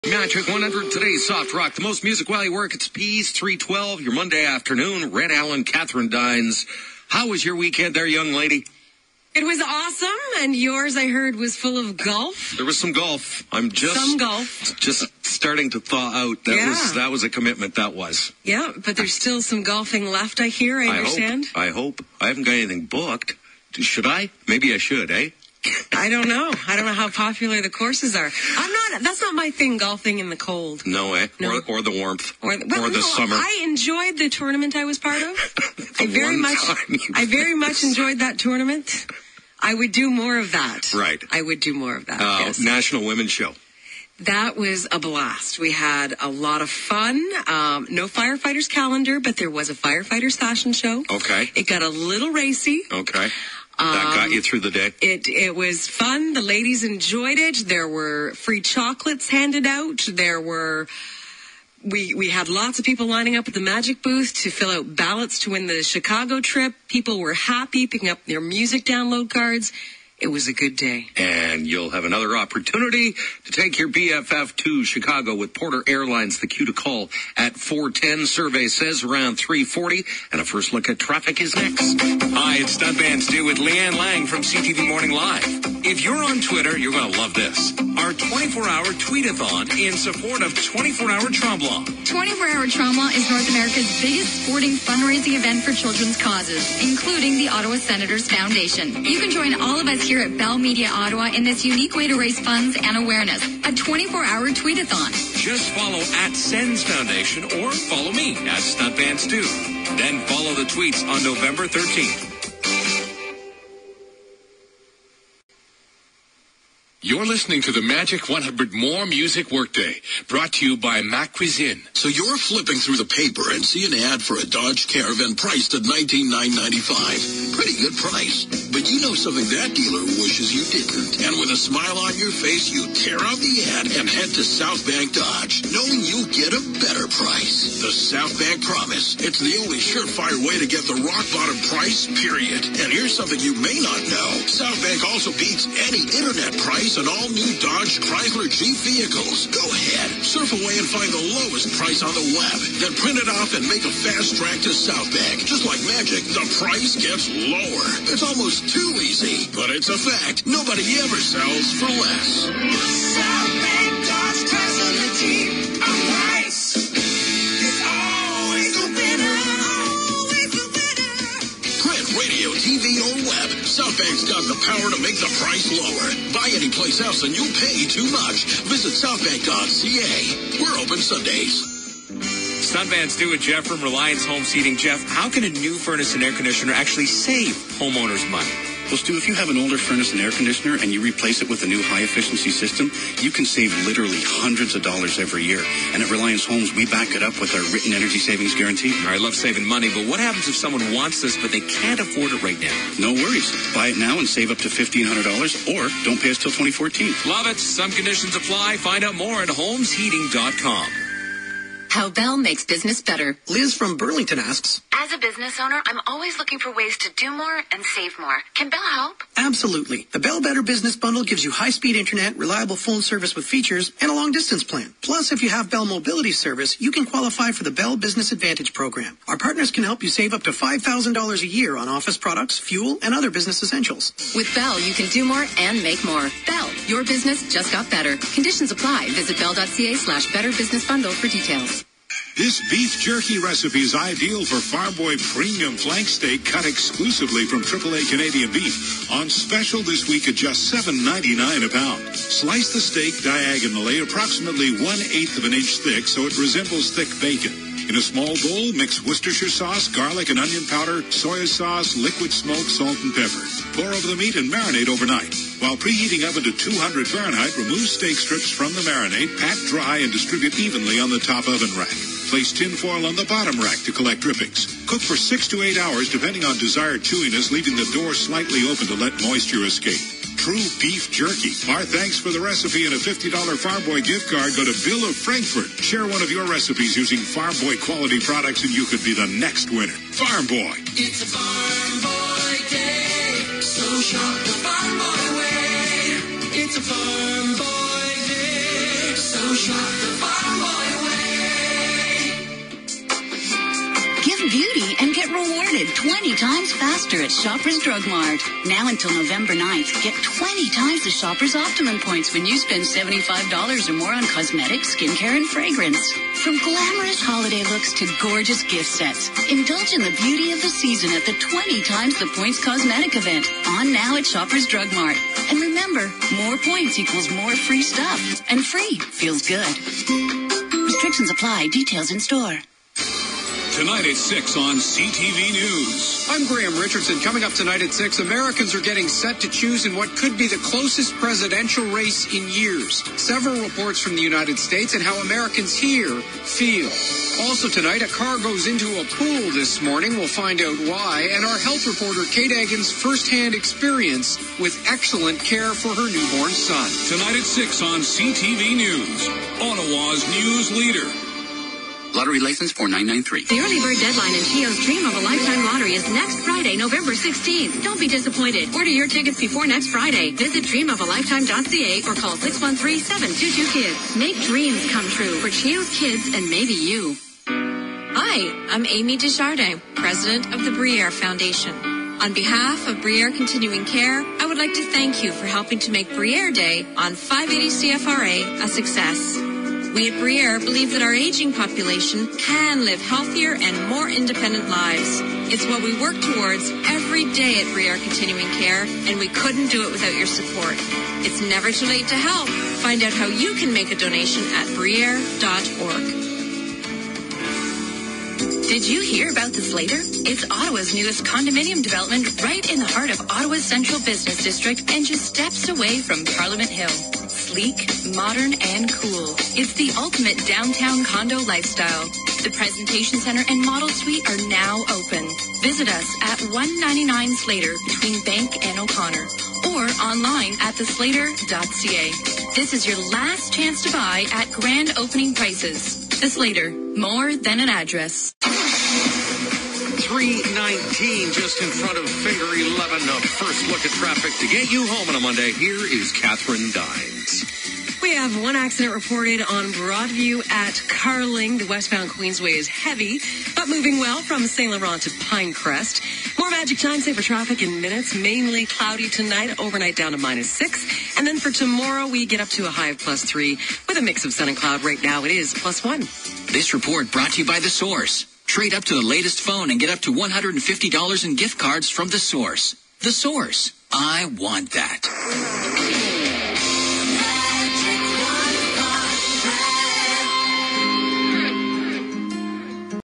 Majic 100 Commercial Break from November 5, 2012
Here is the Majic 100 commercial break from November 5, 2012.

List:
1. Red & Katherine's Weekend Commitments Announcement
2. Stuntman Stu's 24 Hour Tremblant Tweetathon Commercial
3. Ma Cuisine Sponsor for the Majic 100 More Music Work Day
4. Southbank Dodge Chrysler Jeep Caravan Ad Commercial
5. Stuntman Stu's Furnace Holmes Heating Commercial
6. Bell Business Owners Commercial
7. Farm Boy Premium Flank Steak Commercial
8. Shoppers Drug Mart 20x Optimum Points Commercial
9. Ottawa's Newsleader on CTV News Commercial
10. Dream Of A Lifetime Early Bird Deadline for Cheo Kids Commercial
11. The Bair Foundation Successing Bair Day On 580 CFRA Commercial
12. Ottawa's Newest Condominium Development for Grand Opening Prices Commercial